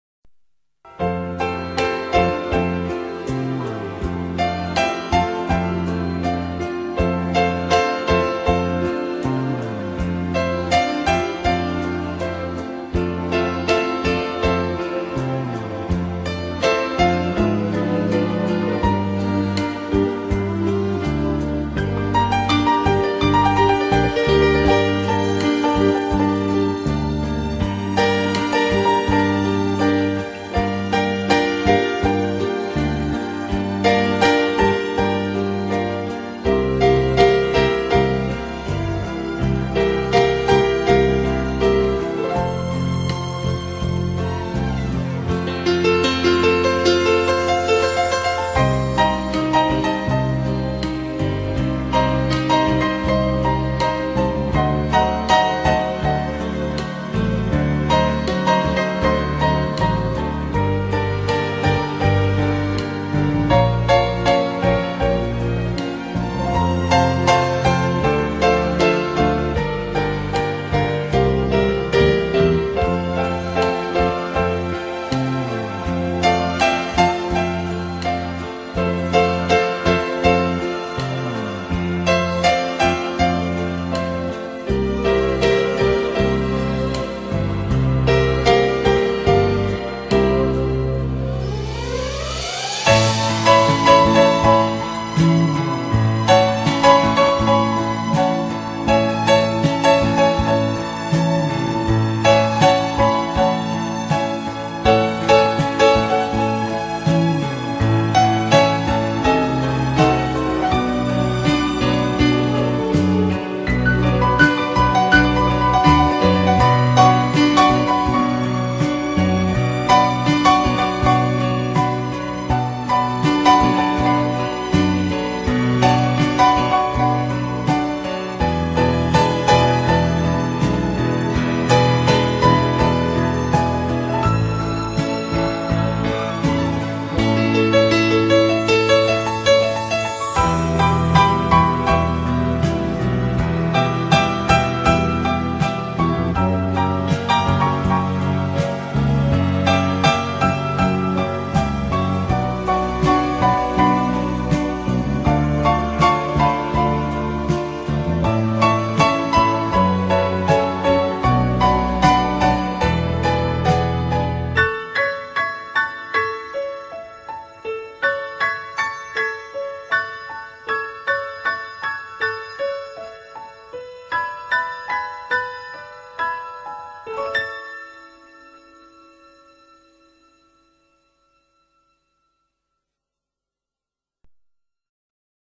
MIRIAM PRATO E LA SUA PITTURA
La musica di accompagnamento è stata eseguita da Richard Clayderman.